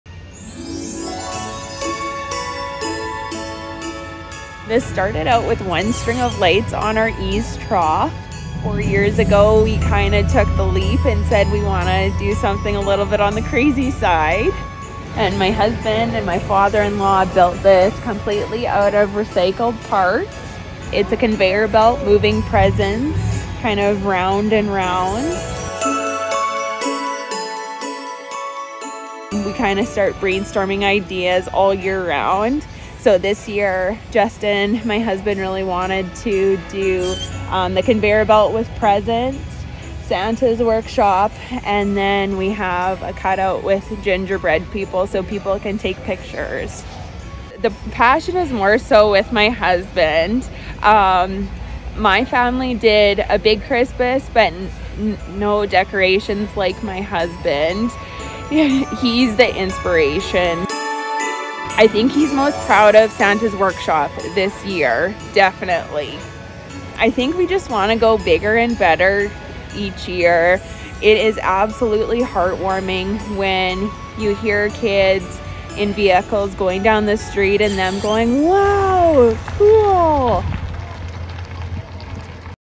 Swift Current couple dazzles with Christmas decorations
Jessica and Justin Perrault want to make their Christmas display "bigger and better" every year. The Swift Current, Sask. couple's house at 267 6th Ave SE has won the city's contest for best decorated home.